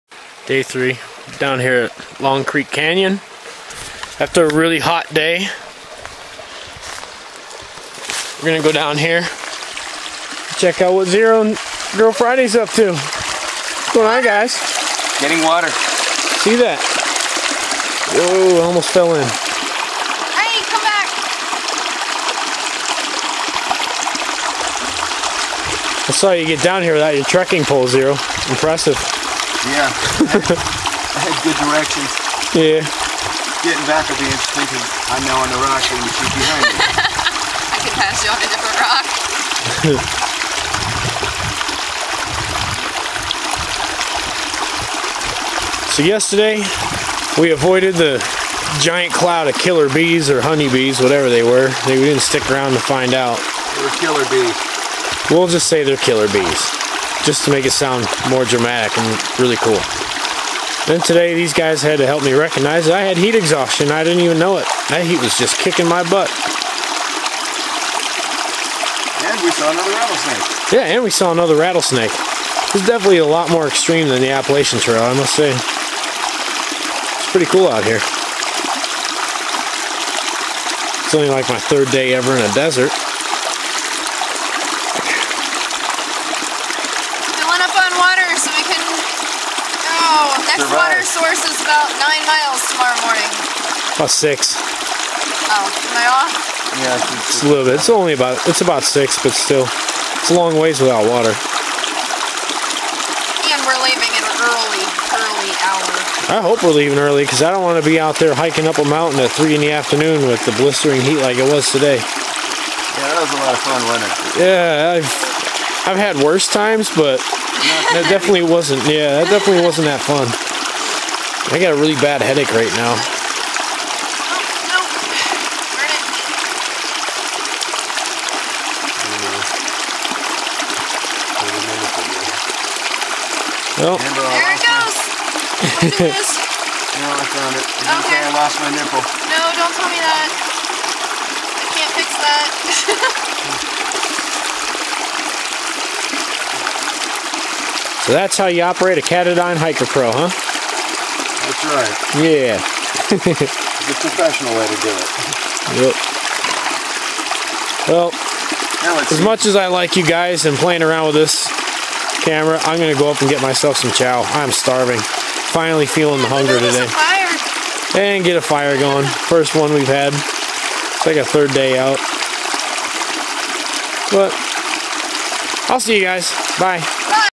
Team Farsight 2010 :: video 12
Day 3- long creek canyon. Refilling the H2O supply & recollecting the killer bee escape as well as Chad's heat exhaustion.